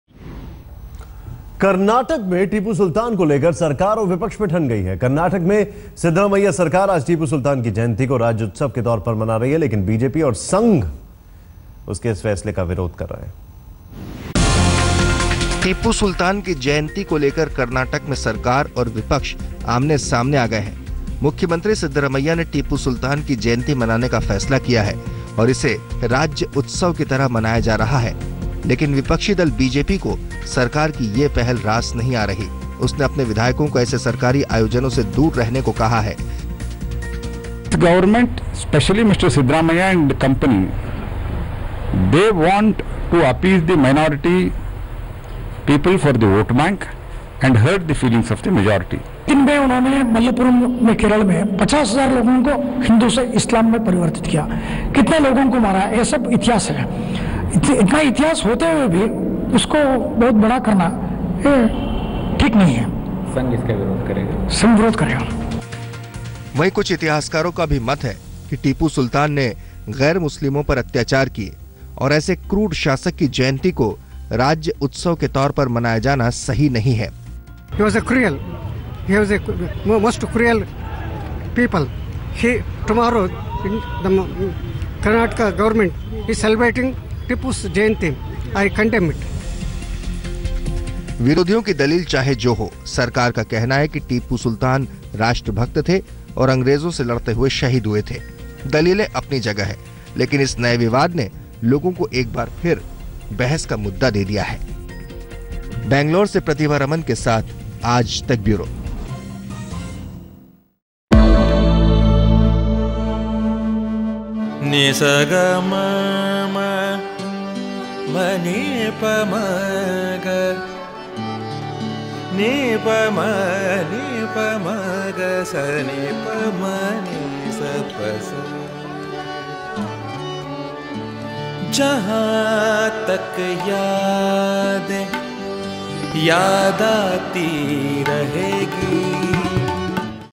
Siddramaiah Govt Faces Protest From Opposition Over Tipu Jayanti
The Siddaramaiah government has faced strong protest from the opposition over the celebration of Tipu Sultan's birth anniversary.